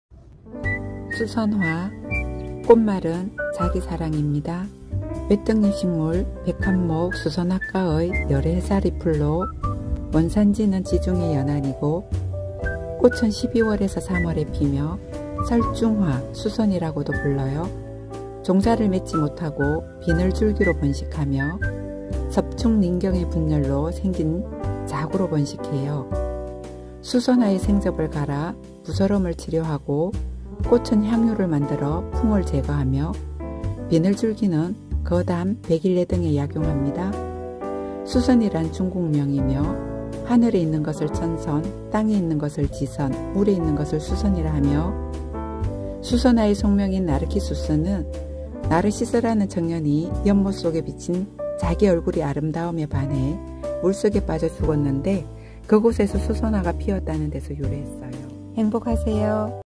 수선화, 꽃말은 자기사랑, 나르시스라는 미소년의 전설에서 유래~ /몽깨[행복시선(視禪)
수선화
Narcissus

생물학적 분류계 : 식물계(Plantae)
문 : 현화식물문(Anthophyta)
강 : 외떡잎식물강(Monocotyledoneae)
목 : 백합목(Liliales)
과 : 수선화과(Amaryllidaceae)
속 : Narcissus크기10~50cm원산지지중해연안, 동북아시아분포지한국, 중국, 일본, 지중해

특징

수선은 비늘줄기에 속하는 내한성이 강한 가을심기 구근으로 이른 봄에 개화된다. 품종개량은 영국, 네덜란드에서 많이 이루어졌고 최근에는 미국에서도 육성되고 있다. 우리나라에는 화단용으로 일부 이용되고 있는 방울수선의 일종인 제주수선이 있다.

한국, 중국, 일본 및 지중해 연안에 자생하고 있으며, 가을에 심는 비늘줄기로서 해마다 갱신되지 않고 구근(알뿌리)의 내부에 인편이 생겨서 점차 비대된다. 구근(알뿌리)의 외부는 얇은 막으로 묶은 인편이 둘러싸고 있으며 내부는 저장양분을 가지고 있는 여러 개의 인편이 중첩되어 중심부를 둘러싸고 있다.

재배정보

1) 심는 방법

이랑은 160~170cm 폭에 60~70cm 통로를 만들고 저온 처리가 끝난 구근을 바로 심되 10~15cm 간격으로 2줄로 심는다. 구근의 상부가 2~3cm 묻히게 심는다.

(1) 가꾸기 포인트

정식할 때 충분히 부숙하지 않은 퇴비를 넣으면 병해의 발생의 원인이 되므로 주의한다.

2) 기후 및 토양

(1) 기후조건

생육적온은 야간 13~15℃이고, 주간은 야간보다 5~10℃ 높게 유지한다. 빛이 부족하면 잎과 꽃대가 웃자라서 연약하고 꽃대보다 잎의 길이가 길어지게 된다.

(2) 토양조건

토양은 사질양토이고 배수 및 보수력이 양호하여야 한다. 토양산도는 pH 6~7이며 염류농도가 낮아야 한다.

3) 관리하기

(1) 물주기

정식 후 뿌리가 내리는 초기에는 충분히 관수해야 하나 뿌리가 충분히 내린 후에는 관수량을 줄여서 잎이 웃자라서 쓰러지지 않고 강건하도록 관리 한다.

(2) 거름주기

밑거름으로 10a당 질소, 인산, 칼리 18~20kg을 뿌리고 30~60cm 정도로 깊게 경운한다.

(3) 질병관리

ㆍ점무늬병
증상 : 잎끝에서부터 황갈색으로 말라 오그라진다.
치료 : 구근소독을 철저히 한다.

4) 수확하기

구근의 수확 적기는 6월 하순으로, 잎이 노란색이 되면 땅 위로 나온 부분을 잘라내고 구근을 캐어 말린다. 마르면 그물 주머니에 넣어 응달에서 보관한다.

출처: 네이버지식백과(경기도농업기술원)
ㅡㅡㅡㅡㅡㅡㅡㅡㅡ

수선화
Paperwhite, 水仙花

외떡잎식물 백합목 수선화과의 여러해살이풀.

학명: Narcissus tazetta var. chinensis
계: 식물
문:속씨식물
강:외떡잎식물
목:백합목
원산지:지중해 연안
크기:잎 길이 20∼40cm, 나비 8∼15mm

본문

설중화·수선(水仙)이라고도 한다. 지중해 연안 원산이다. 비늘줄기는 넓은 달걀 모양이며 껍질은 검은색이다. 잎은 늦가을에 자라기 시작하고 줄 모양이며 길이 20∼40cm, 너비 8∼15mm로서 끝이 둔하고 녹색빛을 띤 흰색이다.

꽃은 12∼3월에 피며 통부(筒部)는 길이 18∼20mm, 꽃자루는 높이 20∼40cm이다. 포는 막질이며 꽃봉오리를 감싸고 꽃자루 끝에 5∼6개의 꽃이 옆을 향하여 핀다. 화피갈래조각은 6개이고 흰색이며, 부화관은 높이 4mm 정도로서 노란색이다. 6개의 수술은 부화관 밑에 달리고, 암술은 열매를 맺지 못하며 비늘줄기로 번식한다.

수선화의 생즙을 갈아 부스럼을 치료하고, 꽃은 향유를 만들어 풍을 제거한다. 비늘줄기는 거담·백일해 등에 약용한다. 수선이란 중국명이며 하늘에 있는 것을 천선(天仙), 땅에 있는 것을 지선(地仙), 그리고 물에 있는 것을 수선이라고 하였다.

수선화의 속명인 나르키수스(Narcissus)는 그리스 신화에 나오는 나르시스(나르키소스)라는 청년의 이름에서 유래한다. 나르시스는 연못 속에 비친 자기 얼굴의 아름다움에 반해서 물속에 빠져 죽었는데, 그곳에서 수선화가 피었다고 한다. 그래서 꽃말은 나르시스라는 미소년의 전설에서 '자기주의(自己主義)' 또는 '자기애(自己愛)'를 뜻하게 되었다.

출처: 네이버지식백과(두산백과)
ㅡㅡㅡㅡㅡㅡㅡㅡㅡㅡㅡㅡ

수선화
학명 Narcissus tazetta var. chinensis Roem.
생물학적 분류문 : 피자식물문(Angiospermae)
강 : 단자엽식물강(Monocotyledoneae)
목 : 백합목(Liliiflorae)
과 : 수선화과(Amaryllidaceae)
속 : 수선화속(Narcissus)

개화기 12월~3월
꽃색 백색
형태여러해살이풀
크기 꽃대 높이 20~40cm

분포:  한국(제주도)
잎은 늦가을에 자라기 시작하고 4-6개이며 길이 20-40cm, 폭 8-15mm로서 긴 선형이고 끝이 둔하며 백록색을 띠고 두껍다.

꽃은 12-3월에 피며 판통은 길이 18-20mm, 꽃대는 높이 20-40cm이고 포는 막질이며 길이 5-6.5cm이고 꽃봉오리를 감싸며 화경 끝에 5-6개의 꽃이 옆을 향해 달린다. 꽃자루는 길이 4-8cm이고 화피열편은 6개로서 둥글지만 끝이 뾰족하며 길이 14-15mm이고 백색이며 하부는 긴 통상이다. 덧꽃부리는 높이 4mm로서 황색이다. 수술은 6개가 덧꽃부리 밑에 붙어 있고 수술대는 길이 1mm이며 꽃밥은 길이 3mm로서 T자형으로 붙어 있다. 암술대는 덧꽃부리와 길이가 비슷하다. 씨방은 하위이며 녹색이고 3실이다.

열매 : 꽃핀 후 결실치 않으므로 종자의 모양은 불투명하다.

줄기: 검은 껍질에 싸인 달걀모양의 비늘줄기에서 잎이 난다. 끝에 막질의 불염포가 있다.

뿌리: 겹쳐진 비늘줄기는 난상 구형이며 외피는 흑색이고 하부에 백색의 수염뿌리가 다수 난다.

생육환경:  습지에서 잘 자란다.

번식방법:  종자를 맺지 못하며 비늘줄기로 번식한다. 습중린경(襲重鱗莖)의 분열로 생긴 자구(仔球)로 번식한다.

이용방안
화(花)는 水仙花(수선화),비늘줄기는 水仙根(수선근)이라 하며 약용한다.

(1) 水仙花(수선화)
① 성분: 신선한 꽃에는 精油(정유) 0.2-0.45%가 함유되어 있으며 주요성분은 eugenol, benzaldehyde, benzyl alcohol, cinnamic alcohol이다. 일본산의 수선화에는 정유 0.011%가 들어 있다. 꽃에는 또 rutin, isorhamnetin-3-rhamno glucoside(narcissin), carotenoid, citronellol 등이 함유되어 있다.
② 약효: 祛風(거풍), 除熱(제열), 活血(활혈), 調經(조경)의 효능이 있다. 婦人(부인)의 자궁병, 월경불순을 치료한다.
③ 용법/용량: 2.4-4.5g을 달이거나 또는 散劑(산제)로 복용한다. 외용: 짓찧어서 塗布(도포)한다.

(2) 水仙根(수선근)
① 봄, 가을에 뿌리를 캐어 苗莖(묘경), 수염뿌리를 제거하고 泥土(이토)를 씻어내고, 뜨거운 물에 담갔다가 햇볕에 말린다. 縱切(종절)하여 細片(세편)을 만들어 햇볕에 말린다.
② 약효: 消腫(소종), 排膿(배농)의 효능이 있다. 일체의 癰腫(옹종), 瘡毒(창독), 蟲咬(충교)를 치료한다. 외용: 짓찧어서 환부에 바른다.

출처: 네이버지식백과(국립수목원 국가생물종지식정보 : 식물)
ㅡㅡㅡㅡㅡㅡㅡㅡㅡㅡ
수선화
과명:수선화과(Amarylidaceae)
한약명:수선화(水仙花)
분포:외래식물(지중해연안 원산)
성분:정유, eugenol, linalool, cineol 등이 들어있다.
염색체:2n=20-22, 30-32

생물학적특징

다년생 초본, 인경은 도란형

잎은 총생, 선형, 길이 20~40㎝, 폭 8~15㎜, 둔두, 녹백색

꽃은 12~3월 백색 개화, 소화경은 5-6개, 포는 막질, 5-7㎝

화피열편은 6개, 길이 14~15㎜, 부화관은 원통형, 황색, 수술은 6개, 꽃밥은 길이 3㎜

열매는 맺지 못함.

용도

약용-(뿌리, 꽃) 종기, 번열증 등에 사용한다.

출처: 네이버지식백과(생물자원정보 - 민속특산식물)
ㅡㅡㅡㅡㅡㅡㅡㅡㅡ
◇ 유익한 영상이면 ☆구독☆ 좋아요